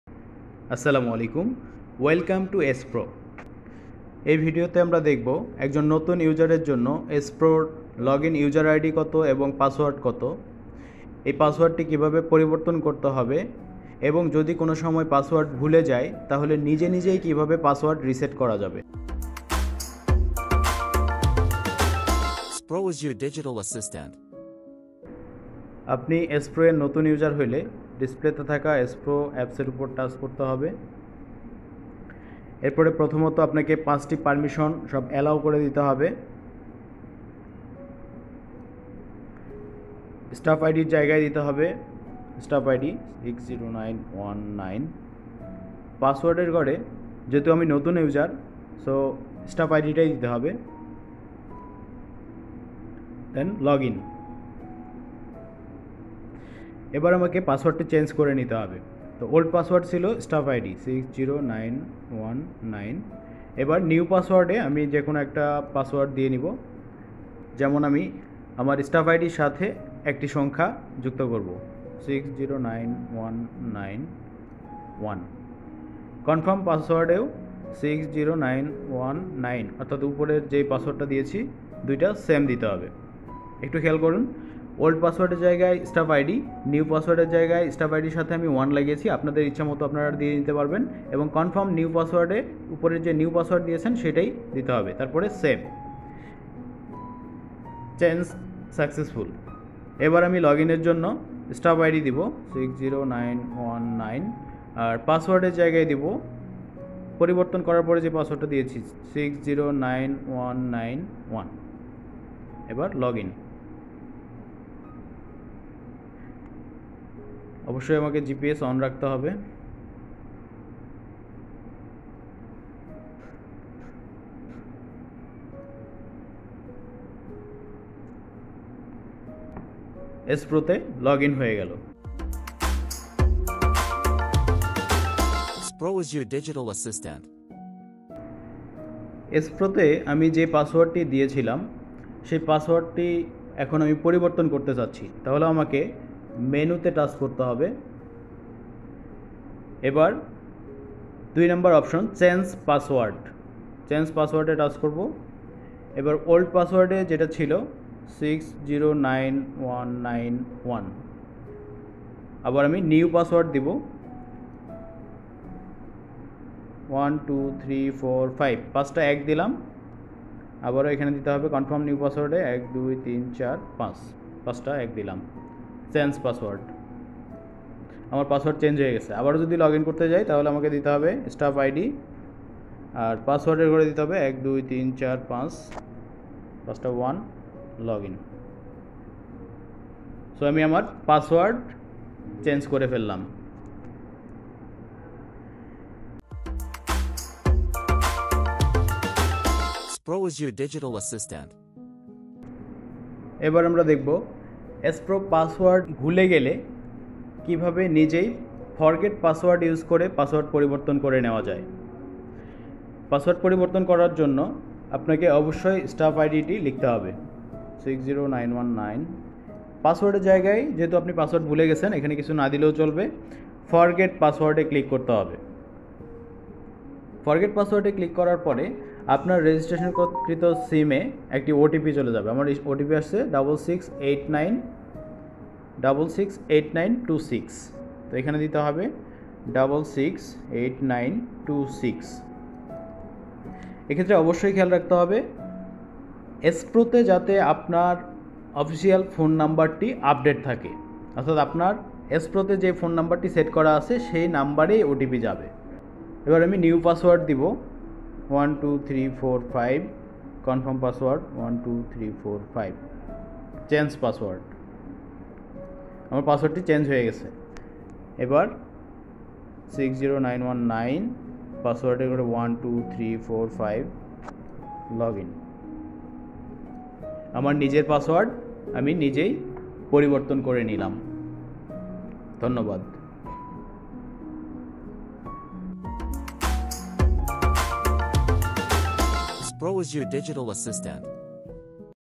How to Change SPRO Password
How to Change Your SPRO Password by Own Using 1. Forget Password 2. Change Password Options.